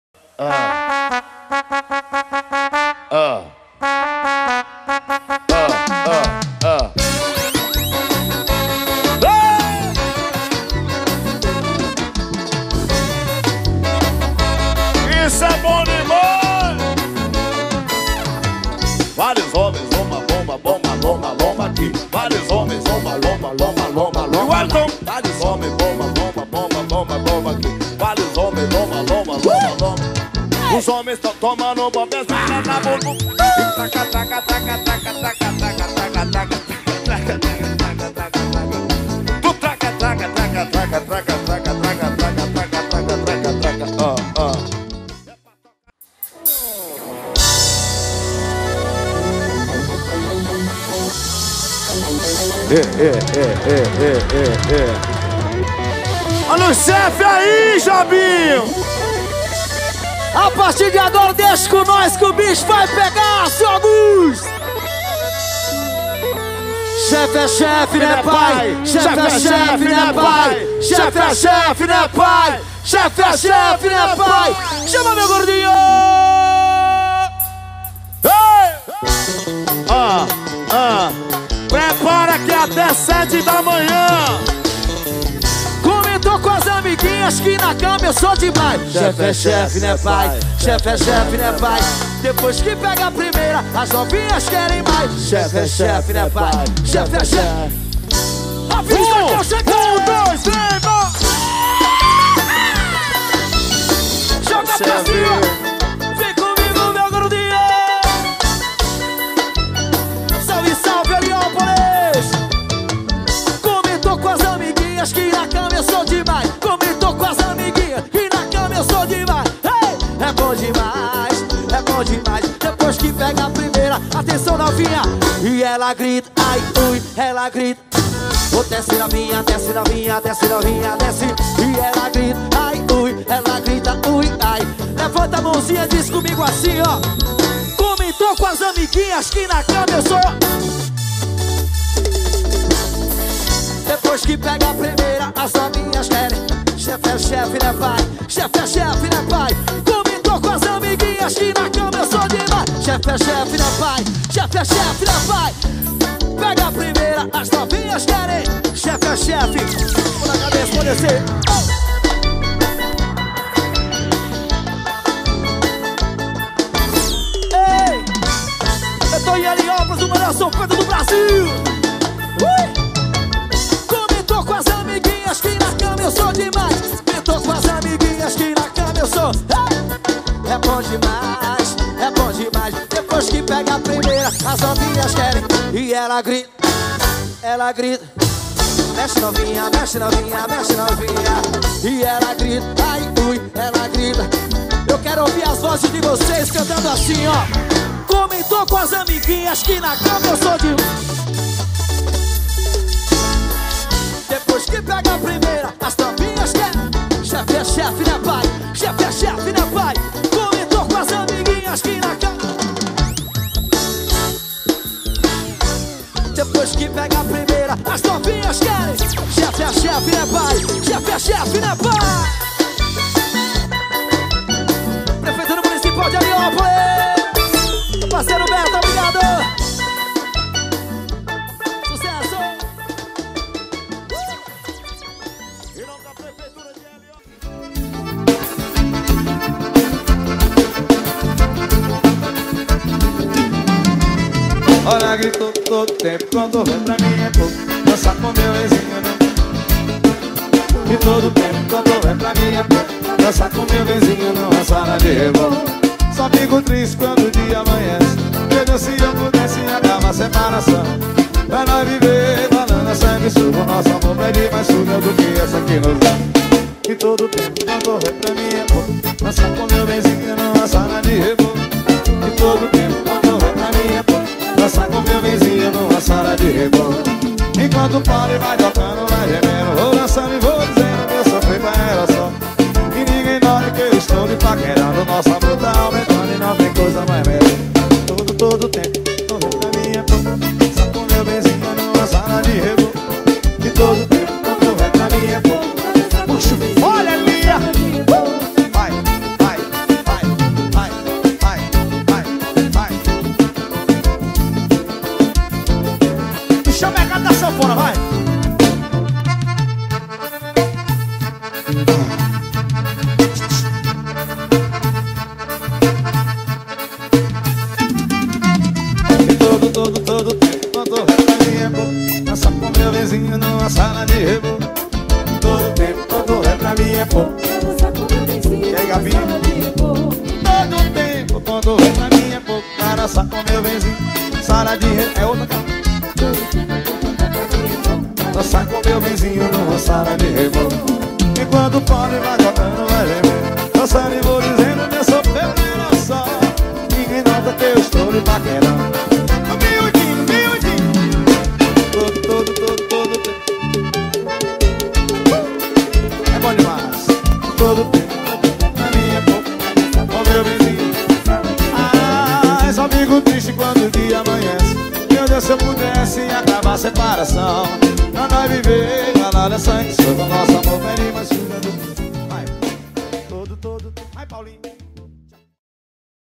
São Pedro de Heliópolis
A tradição da cidade são os festejos juninos em particular para o São Pedro de Heliópolis. Festa tradicional que acontece geralmente no mês de Julho depois das datas tradicionais para comemoração dos festejos juninos.
Alvorada, encontro de paredões, shows ao vivo e muita descontração.